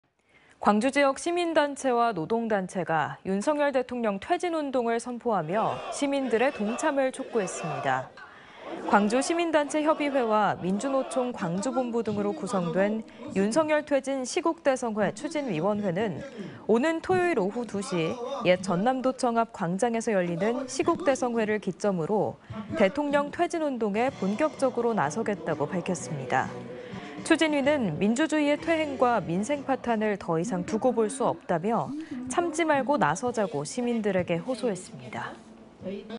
시민단체 '윤석열 대통령 퇴진 운동' 선포
광주지역 시민단체와 노동단체가
윤석열 대통령 퇴진 운동을 선포하며
시민들의 동참을 촉구했습니다.

광주시민단체협의회와
민주노총 광주본부 등으로 구성된
'윤석열 퇴진 시국대성회 추진위원회'는
오는 토요일 오후 2시,
옛 전남도청 앞 광장에서 열리는
시국대성회를 기점으로
대통령 퇴진 운동에
본격적으로 나서겠다고 밝혔습니다.

추진위는 민주주의의 퇴행과 민생 파탄을
더 이상 두고볼 수 없다며
참지 말고 나서자고 시민들에게 호소했습니다.